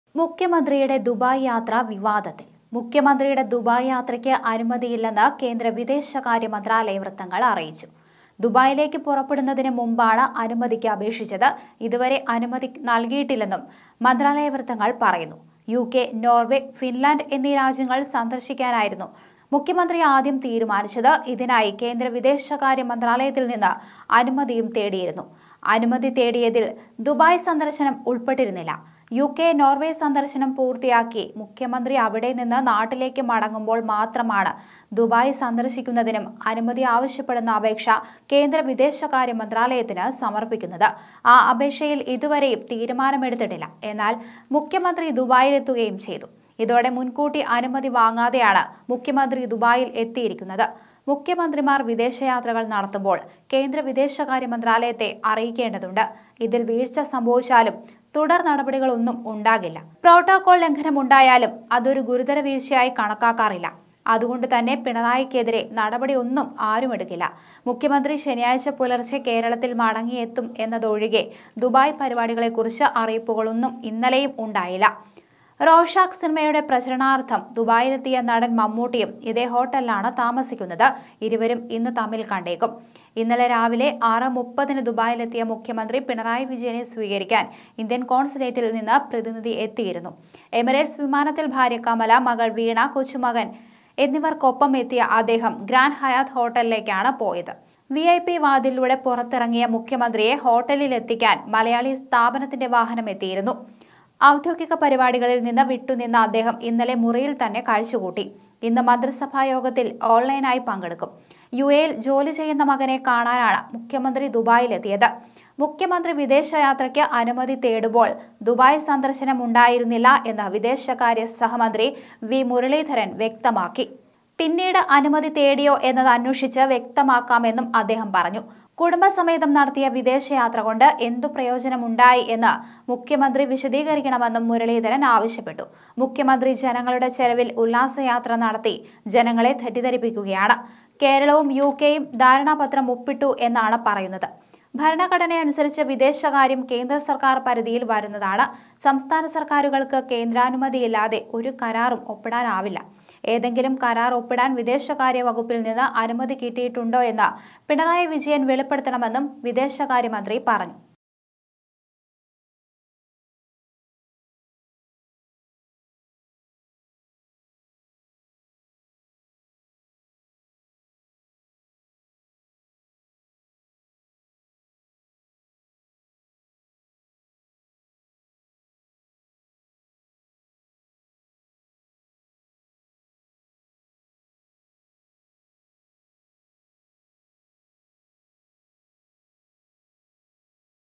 മുഖ്യമന്ത്രിയുടെ ദുബായ് യാത്ര വിവാദം തുടരുമ്പോള്‍ | pinarayi vijayan dubai visit
മുഖ്യമന്ത്രിമാർ വിദേശ യാത്രകൾ നടത്തുമ്പോൾ കേന്ദ്ര വിദേശകാര്യ മന്ത്രാലയത്തെ അറിയിക്കേണ്ടത് കീഴ് വഴക്കം; ഇതിൽ വീഴ്ച സംഭവിച്ചാലും തുടർ നടപടികളൊന്നും ഉണ്ടാകില്ല; പ്രോട്ടോകോൾ ലംഘനമുണ്ടായാലും അതൊരു ഗുരുതര വീഴ്ചയായി കണക്കാക്കാറുമില്ല; മകനെ കാണാനെത്തിയ പിണറായിക്ക് ഒന്നും സംഭവിക്കില്ല; മുഖ്യമന്ത്രിയുടെ ദുബായ് യാത്ര വിവാദം തുടരുമ്പോൾ